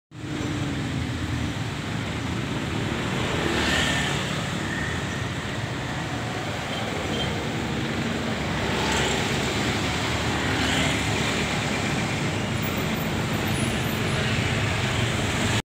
Usaha Jaya
Bus Usaha Jaya Pati-Semarang-pp depan pasar bintoro demak